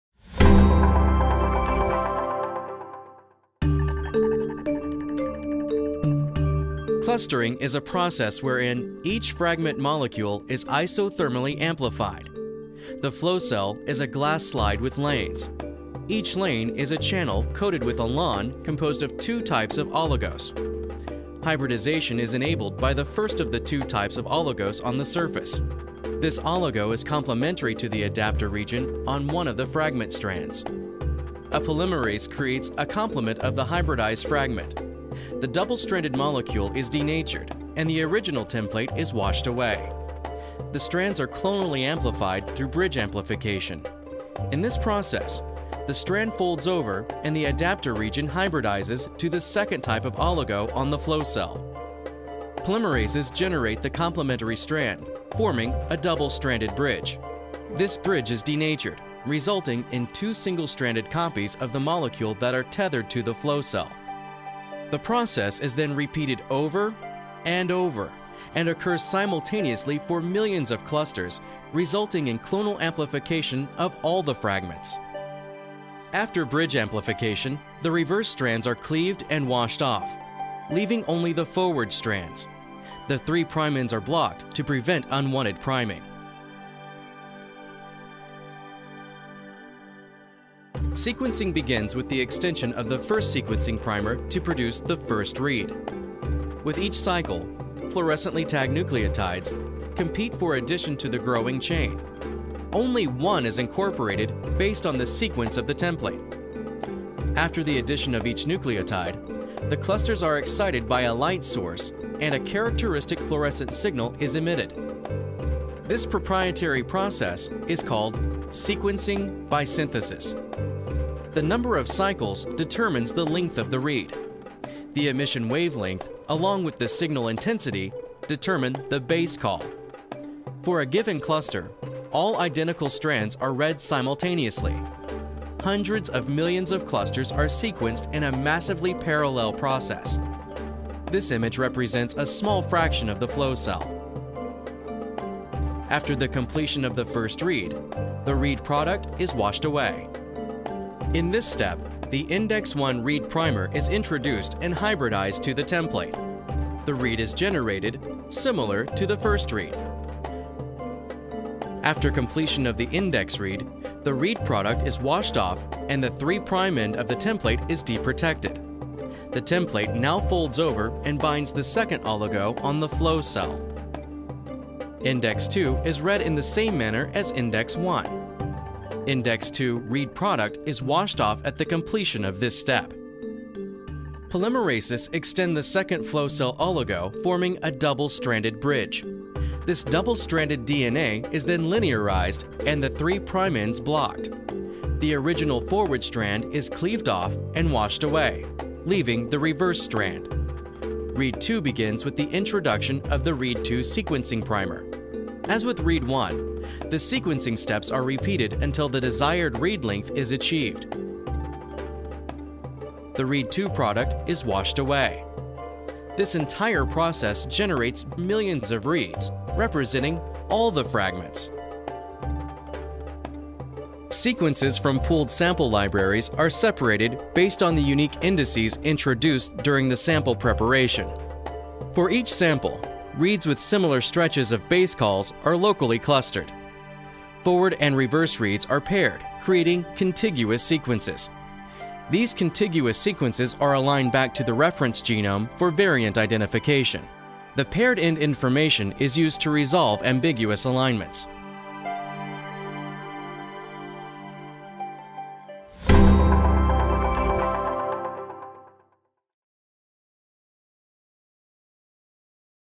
Intro to Sequencing by Synthesis: Industry-leading Data Quality
This video provides an overview of the DNA sequencing workflow on an Illumina sequencer. The process includes cluster generation on a system flow cell, sequencing with Illumina's proprietary sequencing by synthesis technology, and data analysis.